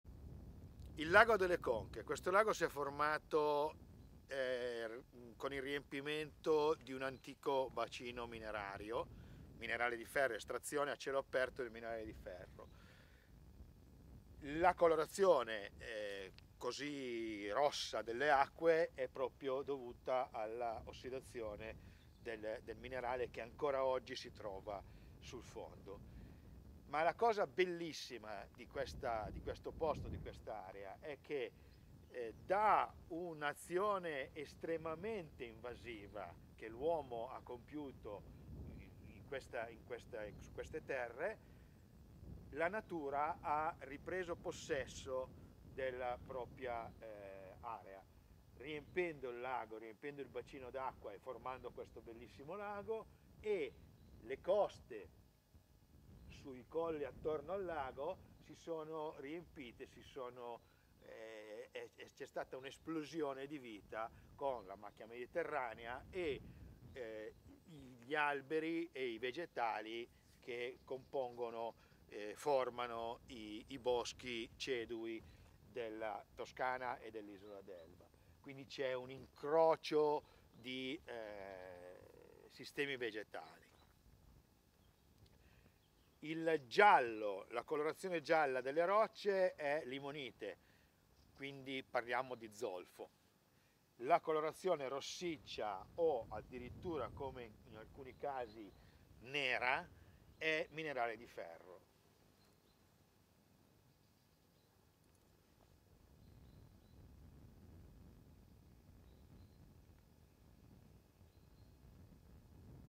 Il Laghetto delle Conche a Rio nell'Elba
Scesi fino alla riva, abbiamo chiesto alla nostra Guida perché il Laghetto delle Conche è così fascinoso, e Simone ci ha detto che..